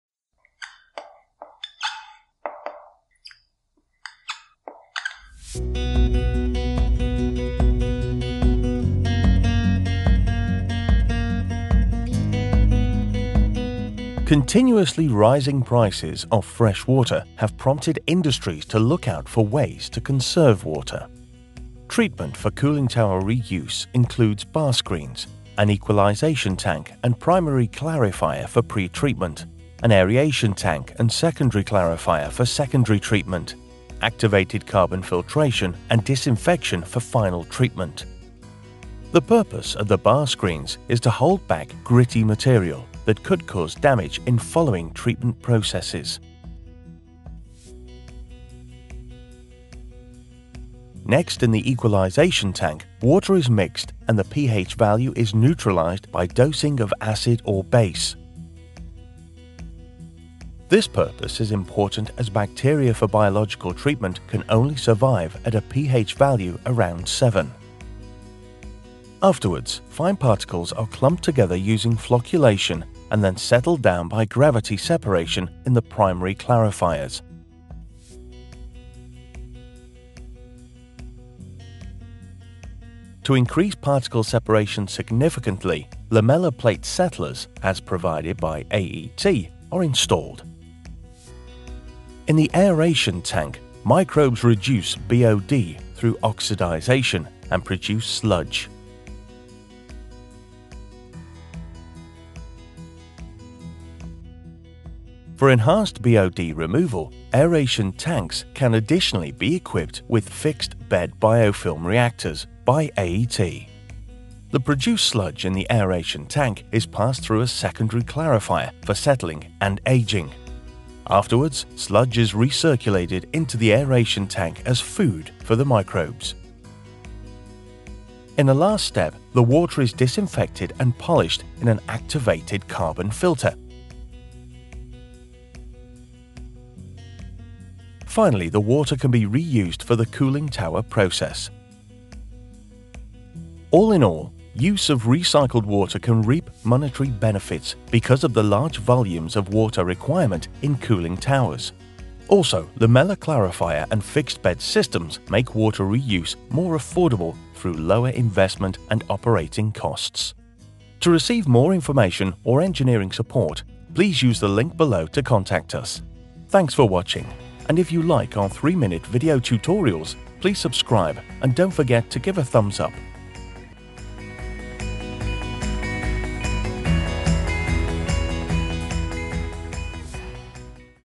Process water reuse - Reduce cost and environmental impact
3 Minute Water and Waste Water Video Tutorials by AET

Rising prices for freshwater let industries look out for ways to reduce cost for process water. One way to reduce cost for process water is water reuse also called water recycling.
To reuse or recycle process water from a cooling tower several steps for water treatment are required.
Bar screens to remove debris that could cause damage in the following treatment steps. In the equalization tank process water is mixed and base or acid is dosed to change the PH value of the waste water. This is important because biological processes in the following steps require a PH value of 7. Afterwards in the lamella clarifier tank particles are removed via sedimentation. Gravity forces let the particles settle down. By dosing flocculants into the waste water particles clump together to bigger particles and settle down faster. This process is called flocculation. By adding lamella clarifier modules aka tube settlers or plate settlers into the lamella clarifier tank the settling velocity of the particles is enhanced. In the next step BOD (biological oxygen demand) in the form of organic waste is removed in the aeration tank. Microbes decompose the BOD into smaller particles which are settled down in the basin as sludge. This sludge is recycled to the lamella clarifier tank where it is aged before it is send back to the aeration tank as food for the Microbes. To improve the aeration process and BOD removal FBBR (fixed bed biofilm reactors) as provided by AET can be added into the aeration tank.
In a final step the process water is polished and disinfected in a activated carbon filter. The activated carbon filter removes the last remaining fine particles before the water can be reused for the cooling tower process. Reuse of process water is becoming more and more important because it reaps monetary benefits but also reduces environmental impact. The lamella clarifier technology aka tube settlers and FBBR (fixed bed biofilm reactors) from AET improve this process and lead to further cost reduction.